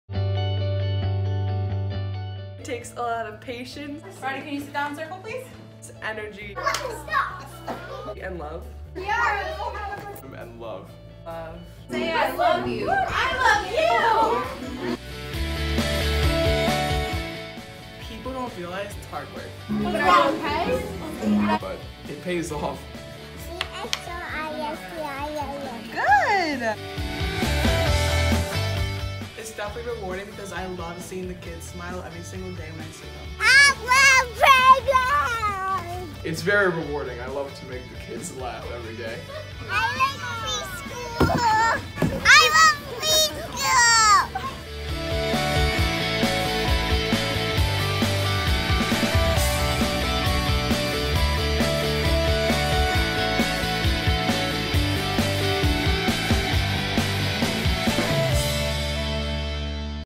Early Childhood Education Project- CTE Video Contest 2018- Entry #5
This video was submitted by David Lopez, Hannah Crump, Riley Southard, Jessica Fiorentino, Shayna Lemus from the Early Childhood Education program at the Ward Technical Center in Riverhead.